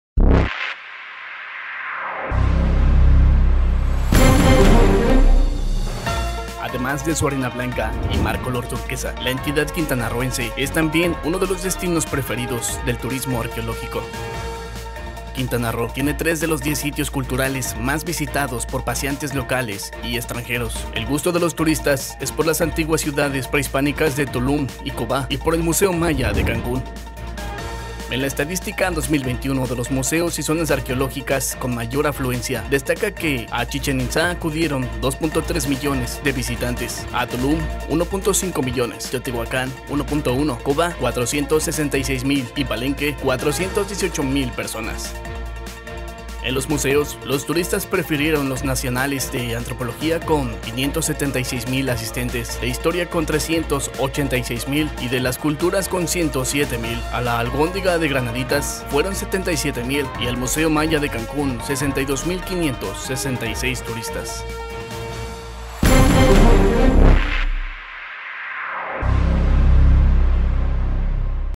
Sobresale Quintana Roo en turismo cultural
Además de su arena blanca y mar color turquesa, la entidad quintanarroense es también uno de los destinos preferidos del turismo arqueológico, al albergar tres de los 10 sitios culturales más visitados por locales y extranjeros en el País.

El gusto de los turistas fue también por las antiguas ciudades prehispánicas de Tulum y Cobá y del Museo Maya de Cancún que resguarda uno de los acervos más sobresalientes de esa sociedad antigua.